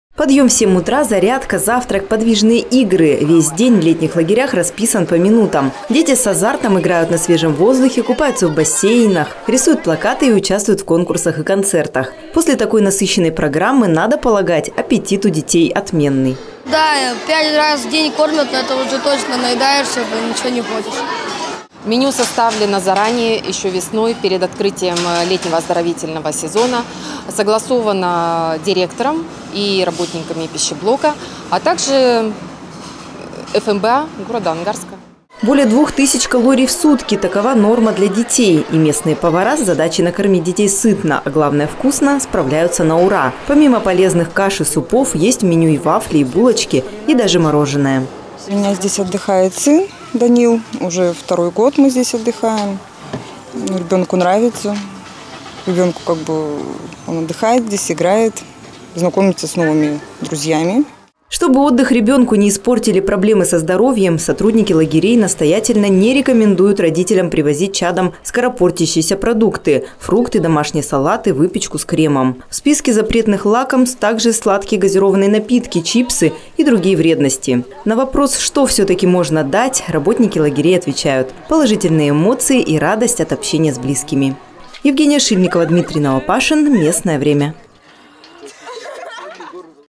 Меню полезное и не очень
Чем питаются дети в летних лагерях, и почему родителям запрещено привозить продукты?